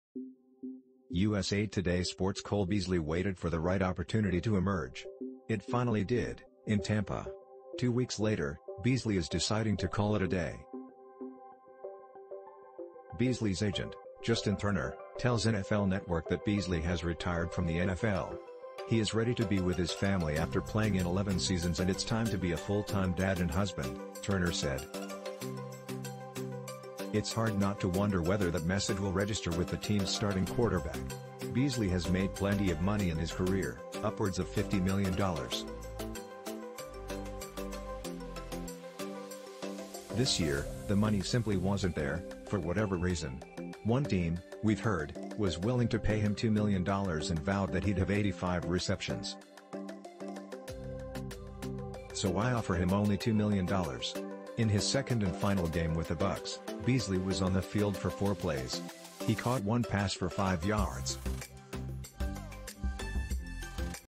Cole Beasley retires from football
USA TODAY SportsCole Beasley waited for the right opportunity to emerge. It finally did, in Tampa. Two weeks later, Beasley is deciding to call it a day. Beasley’s agent, Justin Turner, tells NFL Network that Beasley has retired from the NFL.“He is ready to be with his family after playing in 11 seasons and it’s time to be a full-time dad and husband,” Turner said. (It’s hard not to wonder whether that message will register with the team’s starting quarterback.)Beasley has made plenty of money in his career, upwards of $50 million. This year, the money simply wasn’t there, for whatever reason. One team, we’ve heard, was willing to pay him $2 million and vowed that he’d have 85 receptions. So why offer him only $2 million? In his second and final game with the Bucs, Beasley was on the field for four plays. He caught one pass for five yards.